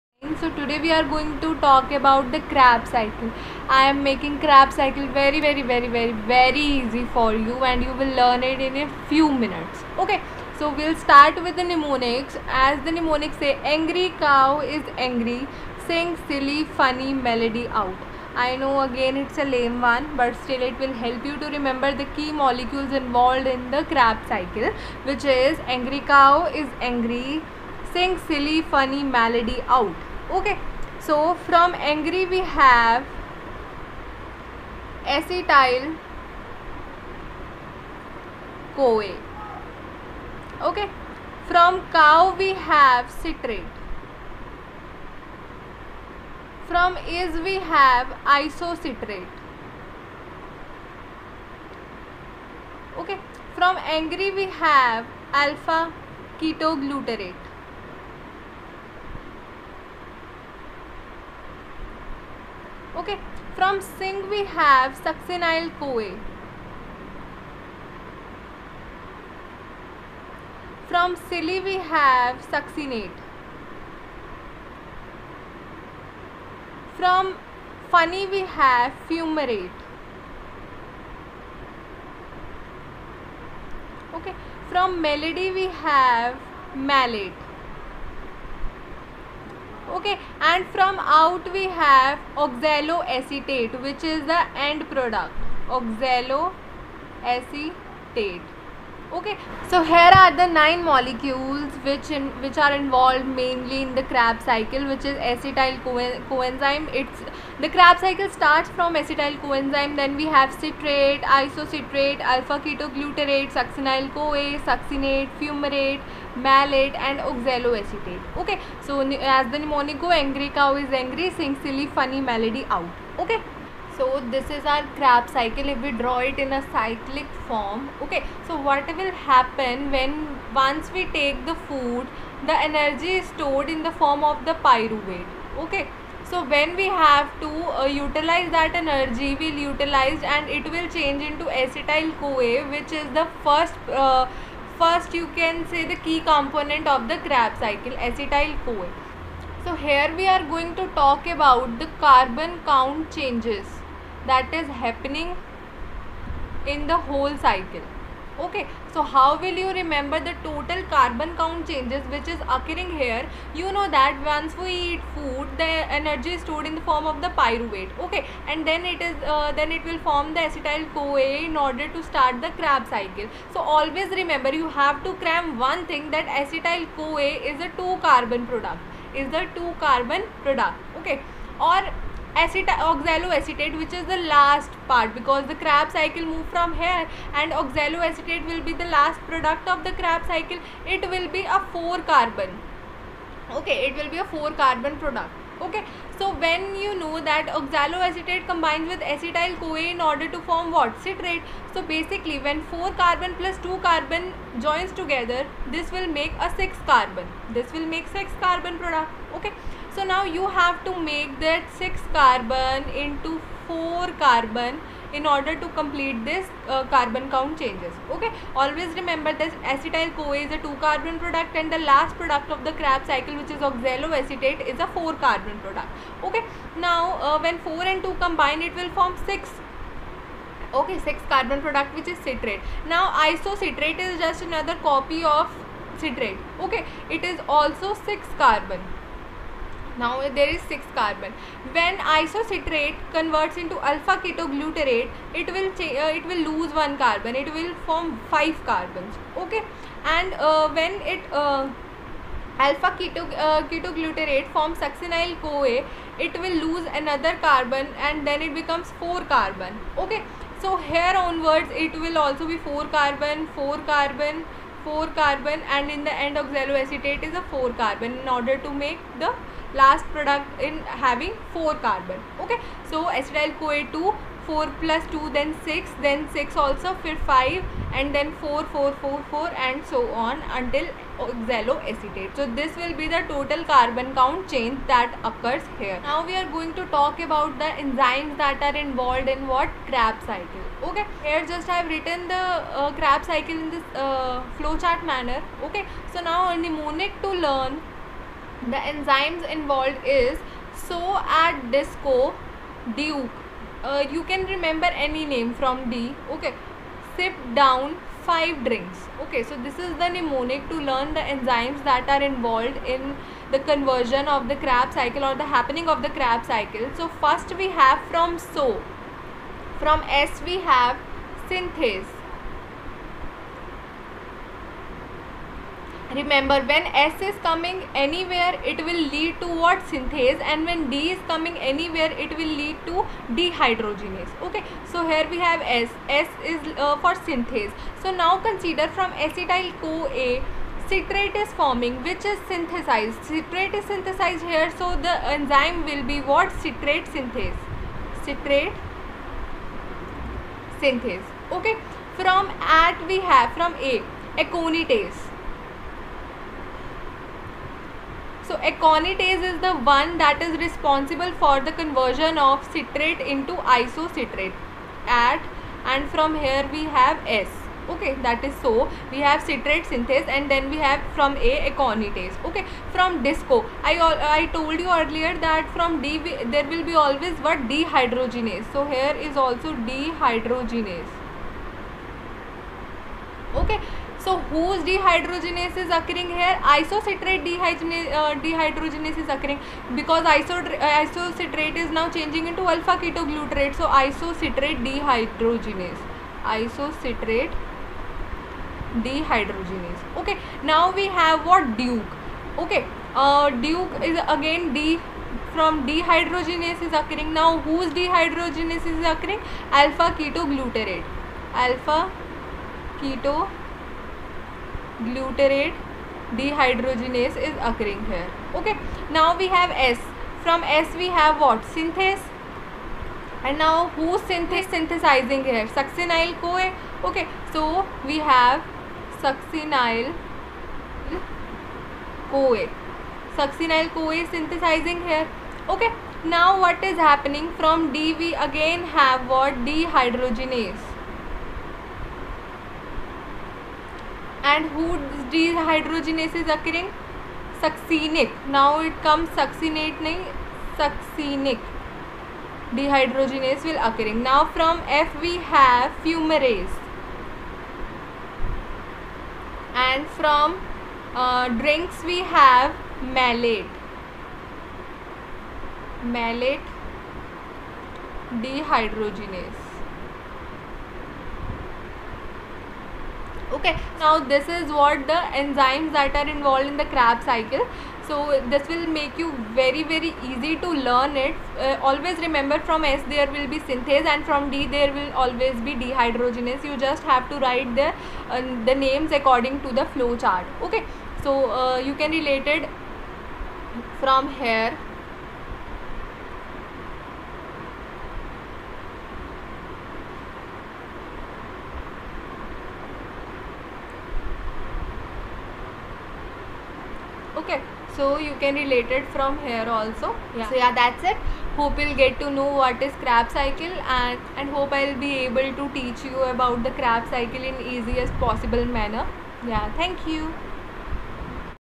Kreb's Cycle | Citric Acid Cycle | Made Easy With Mnemonic
Hello everyone welcome in medicohub worldwide YouTube channel , New video out on kreb's Cycle ® made easy with mnemonics , watch till the end |